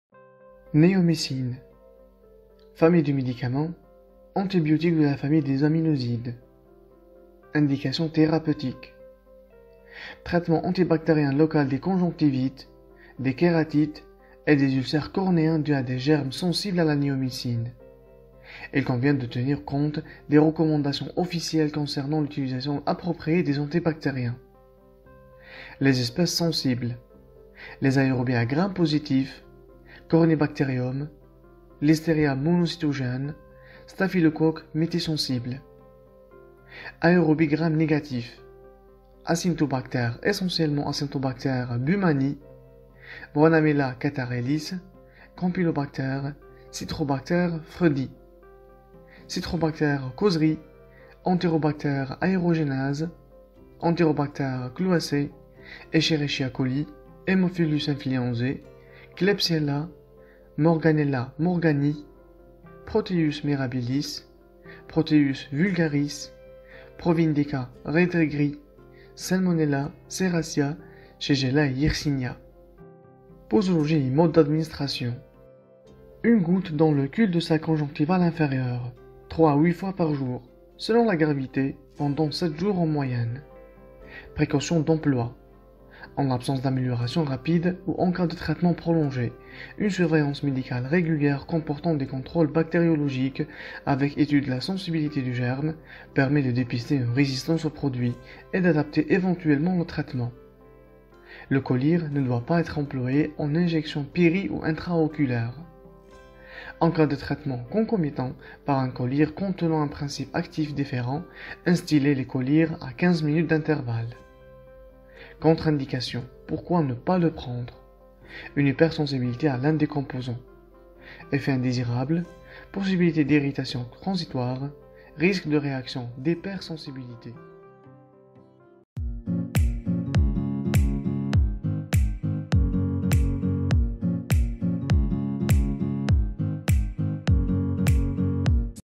NEOMYCINE - TOUT CE QUE VOUS DEVEZ SAVOIR
✅ NEOMYCINE :
** famille du medicament : Antibiotique de la famille des aminosides

Indications thérapeutiques
- Traitement antibactérien local des conjonctivites, des kératites et des ulcères cornéens dus à des germes sensibles à la néomycine.
- Il convient de tenir compte des recommandations officielles concernant l'utilisation appropriée des antibactériens.

Espèces sensibles :
aérobies à Gram + : Corynebacterium, Listeria monocytogenes, Staphylococcus méti-S,
aérobies à Gram - : Acinetobacter essentiellement Acinetobacter baumannii, Branhamella catarrhalis, Campylobacter, Citrobacter freundii,
Citrobacter koseri, Enterobacter aerogenes, Enterobacter cloacae, Escherichia coli, Haemophilus influenzae, Klebsiella, Morganella morganii,
Proteus mirabilis, Proteus vulgaris, Providencia rettgeri, Salmonella, Serratia, Shigella, Yersinia.

Posologie et mode d'administration
1 goutte dans le cul-de-sac conjonctival inférieur 3 à 8 fois par jour, selon la gravité, pendant 7 jours en moyenne.

PRECAUTIONS D'EMPLOI :
- En l'absence d'amélioration rapide ou en cas de traitement prolongé, une surveillance médicale régulière comportant des contrôles bactériologiques
avec étude de la sensibilité du germe, permet de dépister une résistance au produit et d'adapter éventuellement le traitement.
- Le collyre ne doit pas être employé en injection péri ou intra-oculaire.
- En cas de traitement concomitant par un collyre contenant un principe actif différent, instiller les collyres à 15 minutes d'intervalle.